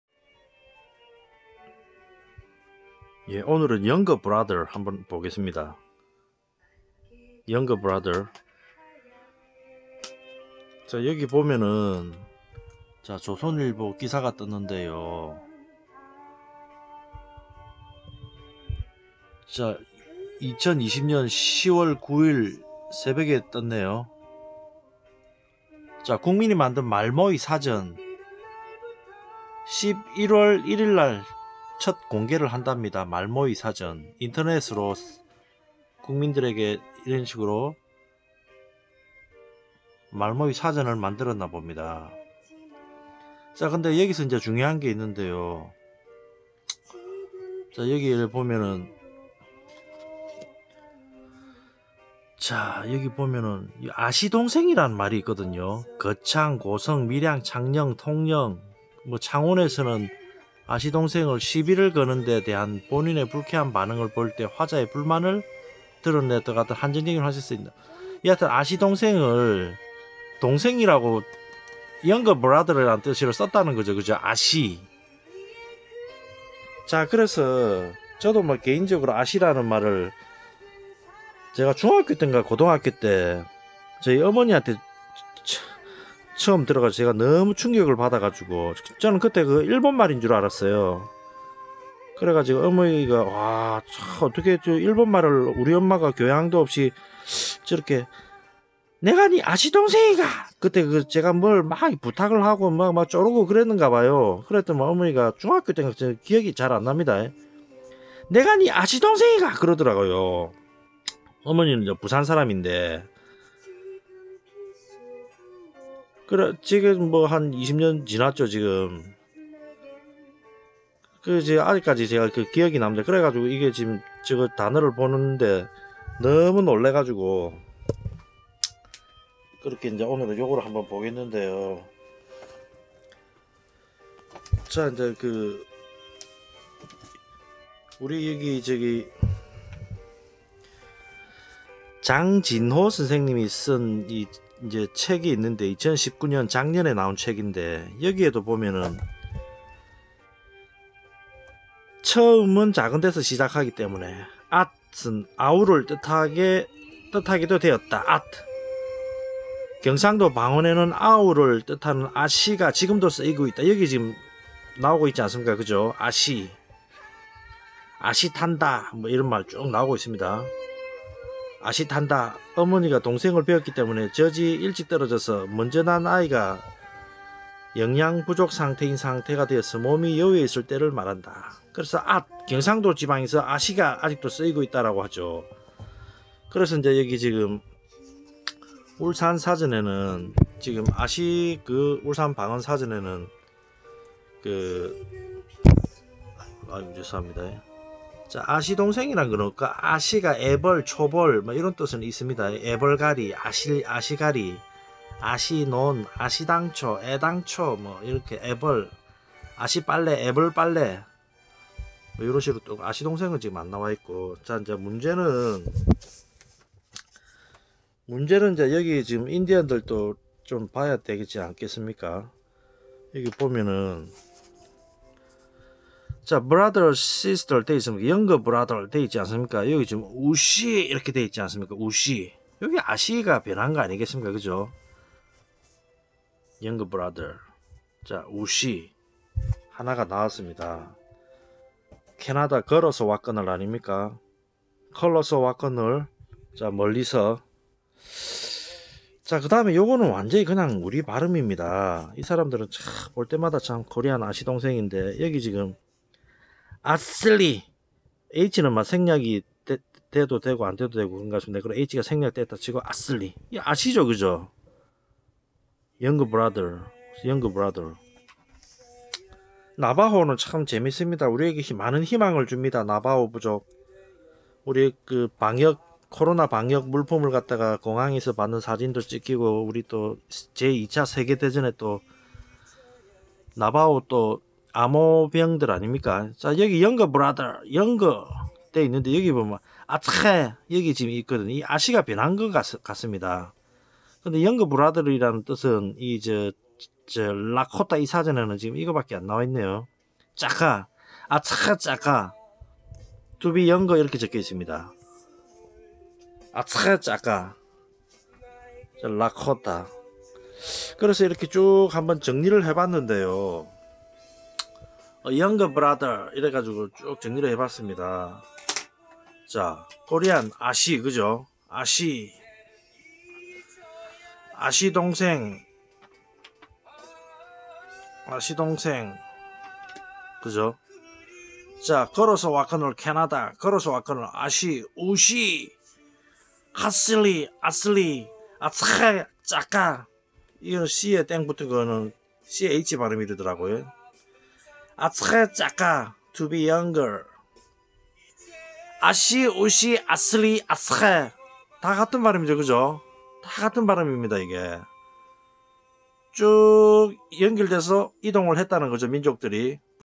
142) a younger brother ;아시동생, Navajo, Lakota, Kolusu Wakonol.
a younger brother ;
Korean ㅡㅡㅡㅡㅡㅡㅡㅡㅡ   ; asi 아시 동생
Navajo   (USA) ㅡㅡㅡㅡㅡㅡ  ; atsili
Lakota   (USA)ㅡㅡㅡㅡㅡㅡ   ; athe
Kolusu Wakonol (CANADA)    ; uhsi

놀랍다.
'아시' 단어 하나로 친숙하게 느껴지는 Canada, Usa 원주민입니다.
아메리카 원주민은 고조선이나 고구려 역사와
땔래야 땔 수 없는 부족임에 틀림없습니다.